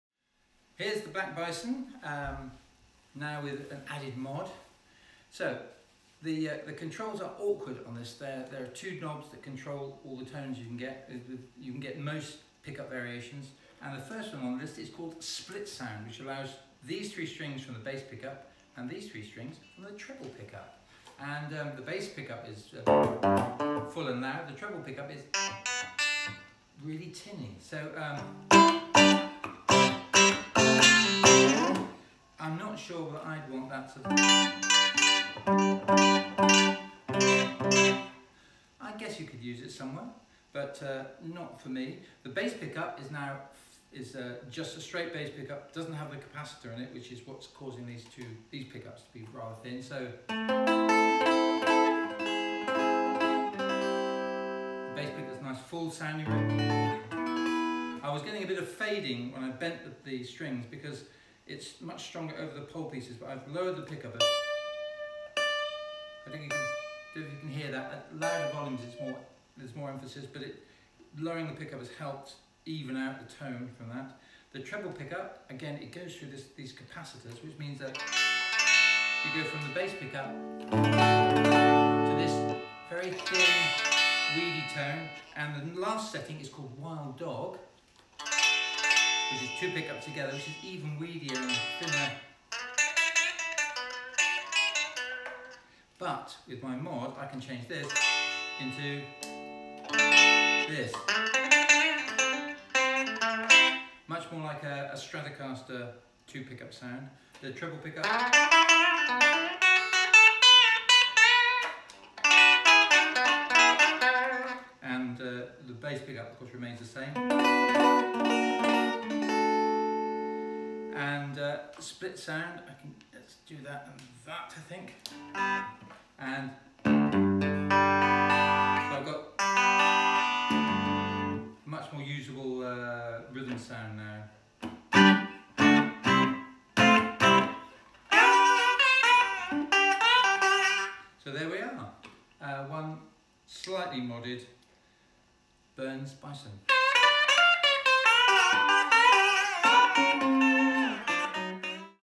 Burns Bison Demo
A demo of all the different pickup combinations.